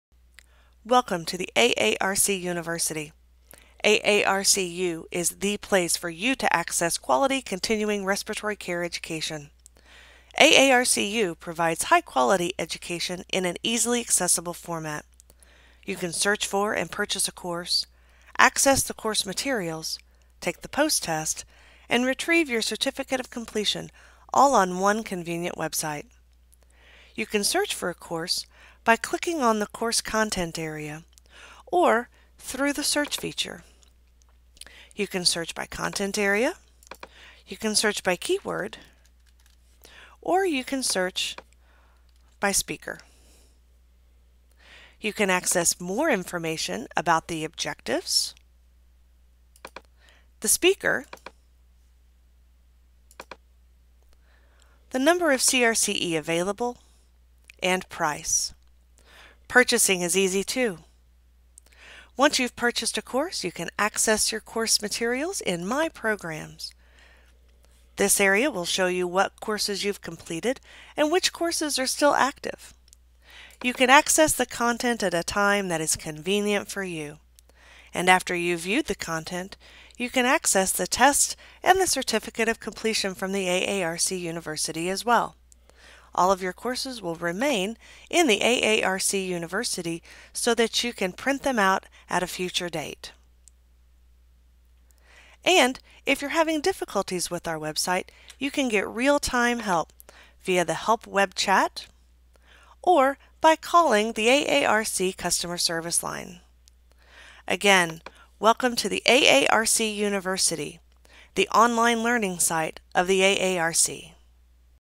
Welcome to AARC University
Shawna Strickland, PhD, RRT-NPS, FAARC, AARC Director of Management and Education, gives a quick preview of AARC University, the online learning site of the AARC.